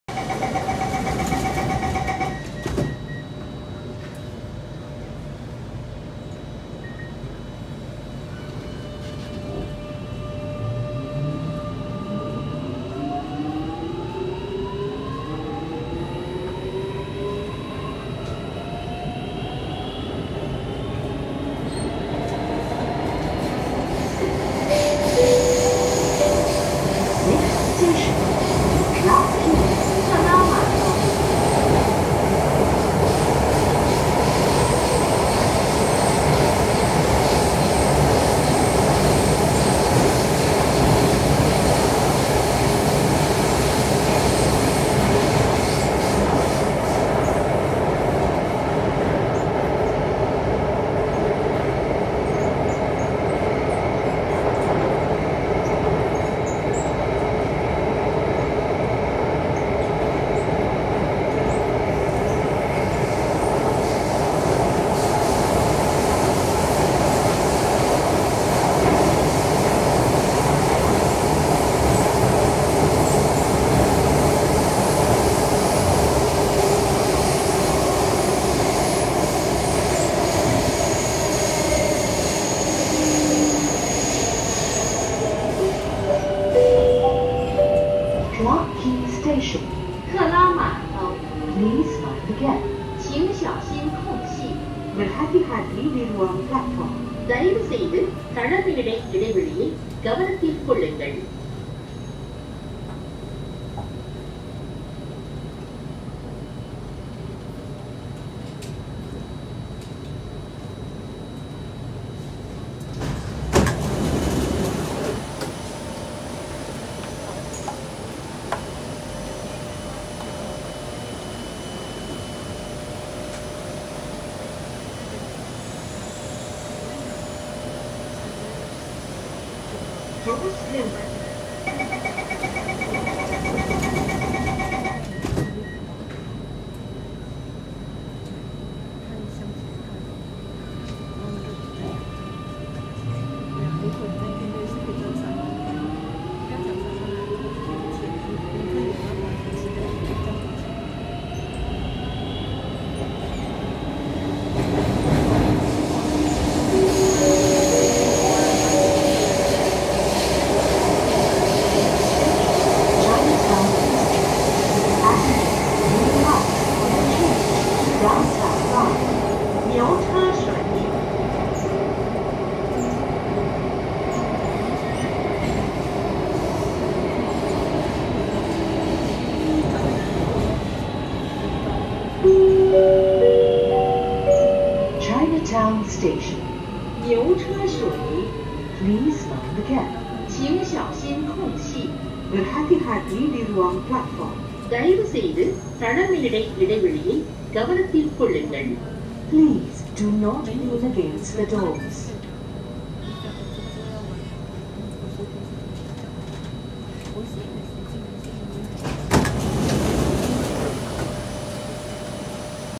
⁴ᴷ⁶⁰ [Unrefurbished C751A] SBST, NEL Train Ride [Dhoby Ghaut → Chinatown] - Alstom C751A 7023/7024
Date of Recording: 23 December 2021
Train: 320
Line to Destination: North East Line to HarbourFront Terminal
Rolling Stock: Alstom Transportation - C751A 7023/7024
Stretch: NE6 NS24 CC1 Dhoby Ghaut to NE4 DT19 Chinatown

Its been a while since I last took NEL and so happened that I rode 1 and got an unrefurbished C751A out. Since the 2nd one is preparing for refurbishment and the completed one is out for test, only the interior would pretty changed much. Similar to the KHI C151 trains though, just that a simple cosmetic change to the exterior. Well, its better to be chasing all the unrefurbished C751A here now because you don't know when the next one will be called for refurbishment.